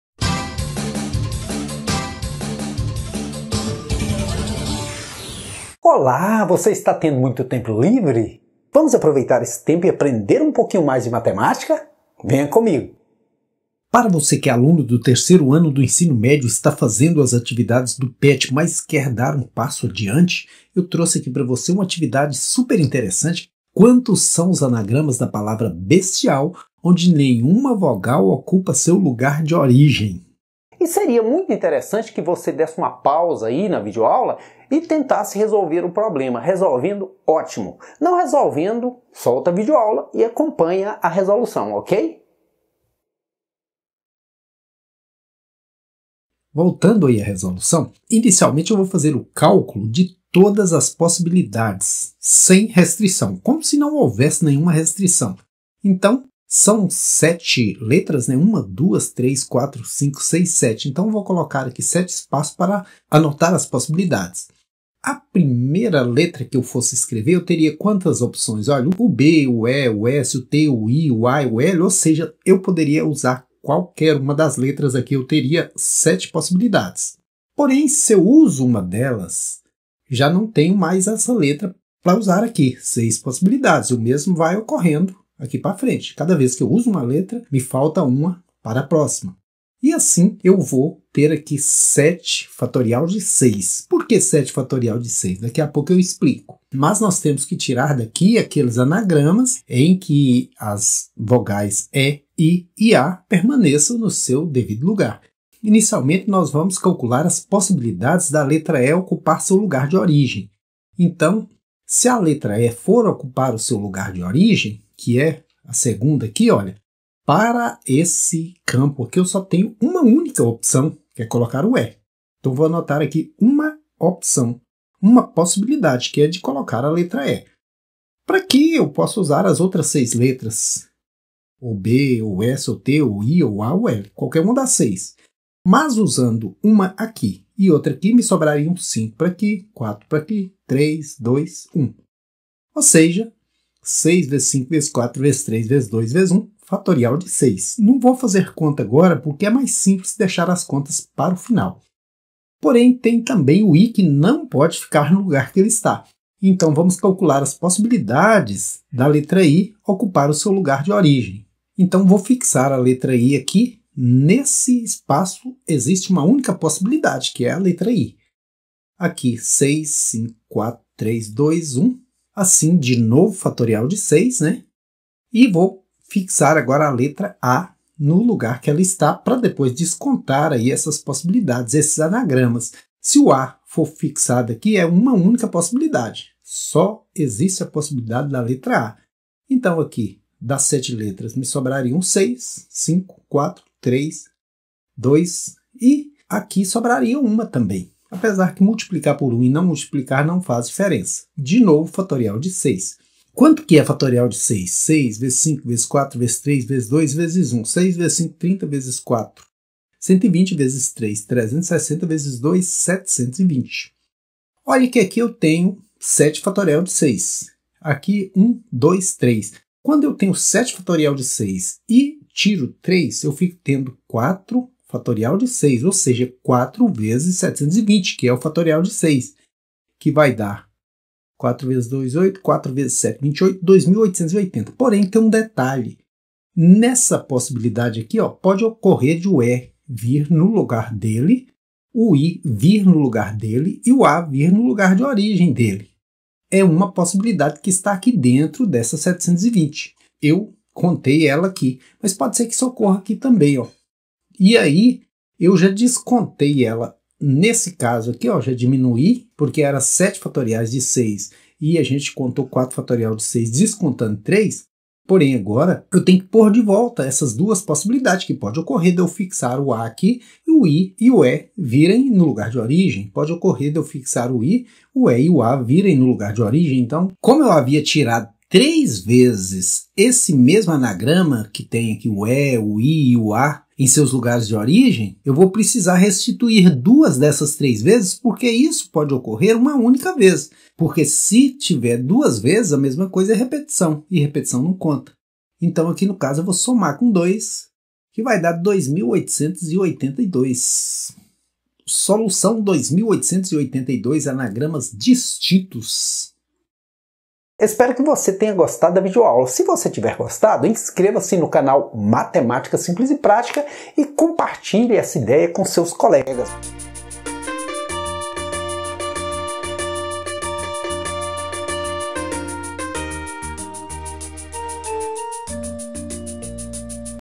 PET VOLUME 2 , QUESTÃO ADICIOANAL (Análise Combinatória)
Para você que busca vídeo aula de Matemática de qualidade, com explicações claras, das Atividades do PET(Plano de Estudo Tutorado) , eu trago, no Canal Matemática Simples & Prática uma atividade complementar, para aprofundamento do tema trabalhado  no PET(Plano de Estudo Tutorado) Volume 02. 3º ano do Ensino Médio